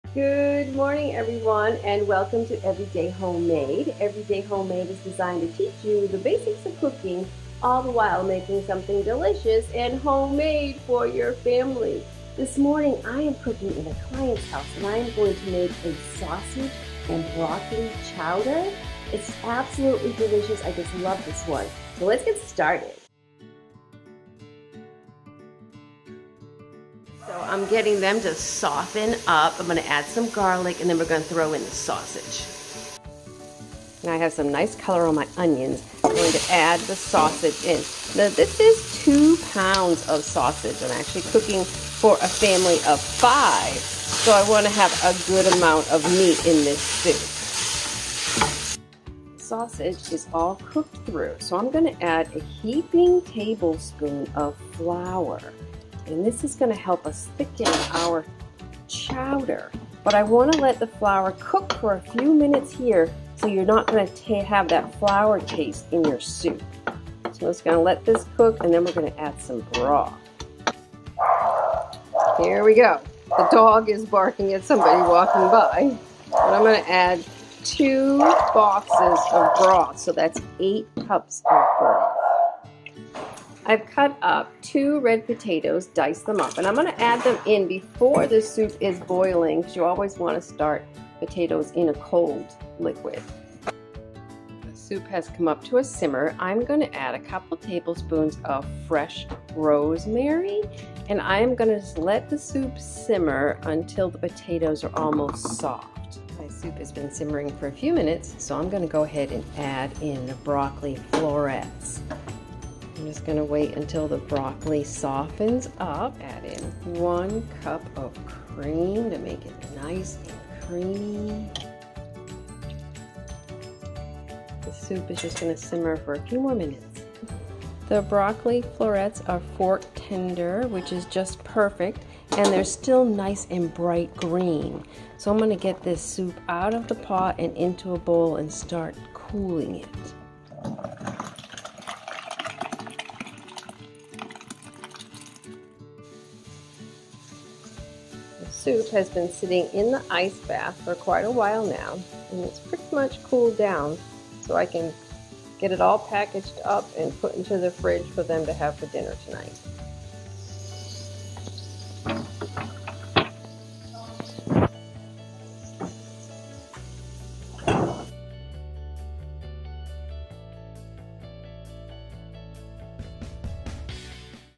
#30 Creamy Sausage and Broccoli Chowder
Well, Hey There!  Chef Donna Here!

Everyday Homemade is designed to teach you the basics of cooking all the while making something delicious and homemade for your family.

In this video I prepare a Sausage and Broccoli Chowder with ground sausage, broccoli florets, with potatoes and fresh herbs!

Creamy Sausage and Broccoli Chowder
Serves 3-4
1 lb ground pork sausage
1 T butter
1 onion, diced
2 garlic cloves, minced
1 heaping tablespoon flour
5 cups chicken stock
2 medium red potatoes, diced
1 tablespoon fresh rosemary
1 broccoli bunch
1 cup cream

In a large pot brown onions and garlic in butter
Add ground sausage, cook until no longer pink
Sprinkle with flour, stir in and cook for two minutes
Pour in the broth scraping up flour on bottom of pot
Add potatoes, and season with salt and pepper
Bring to a simmer and cook for 10 minutes
Add broccoli and cream
Simmer for 5-8 minutes or until potatoes and broccoli are tender